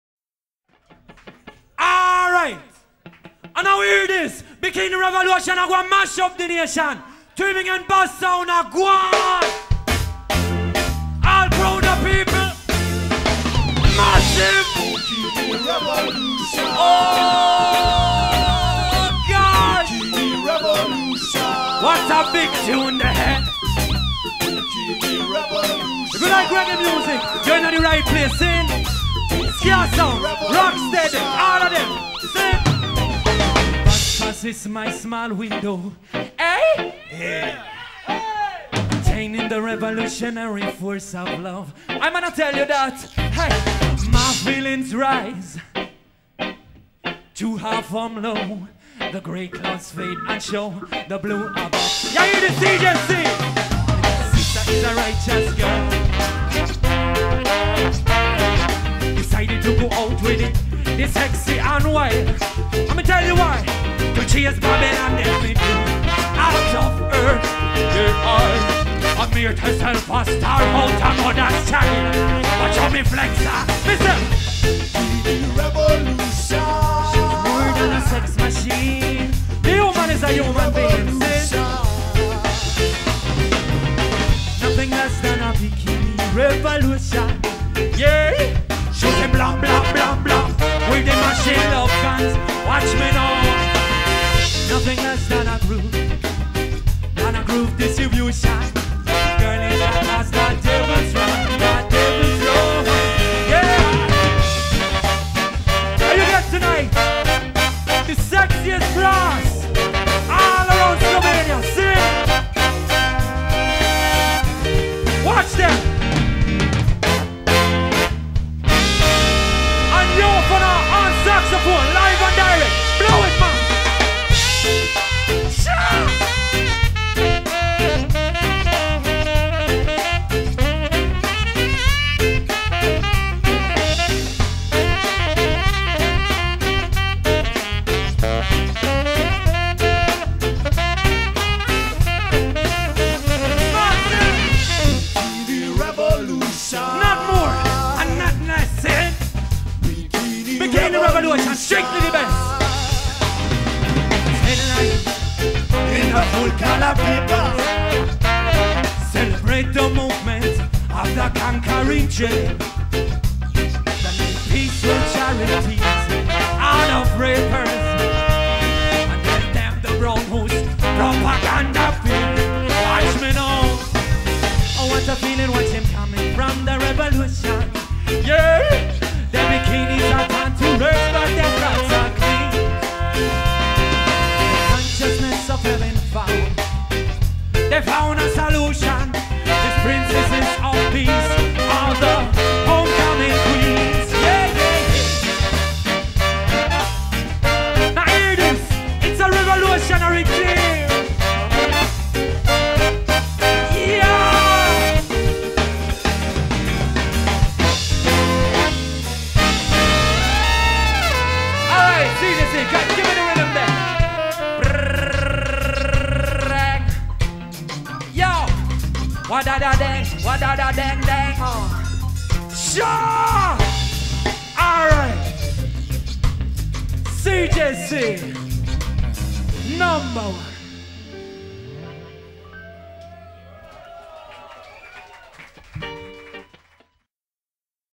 Court Jester's Crew
CJC, very good ska band from Germany, recorded in MKNZ, rock club in Ilirska Bistrica 4.February 2001 (Slovenia, EU).

HawkHome Production